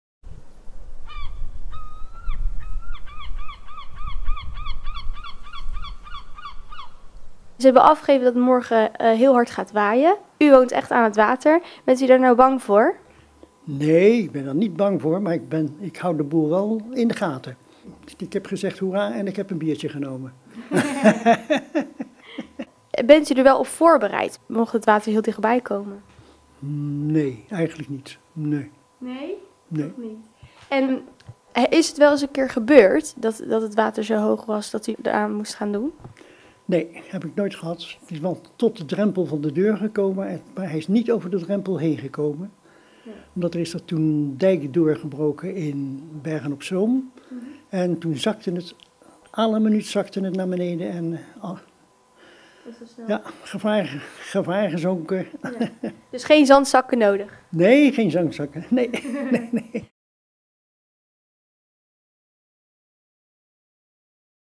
Wonen aan het water, maar geen zorgen voor de storm!
Vrijdagmiddag geven ze code oranje af vanwege storm en hoogwater, maar Theodorus Cleophas uit Krabbendijke die aan het water woont maakt zich geen zorgen!